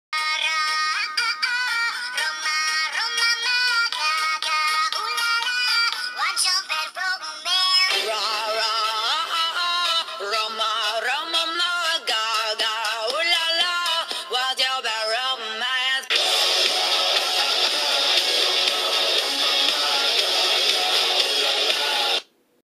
Evolution Of NiceEli 274
these Evolution videos have been a trend I'd say for awhile so I decided to make one and a lot of them use this song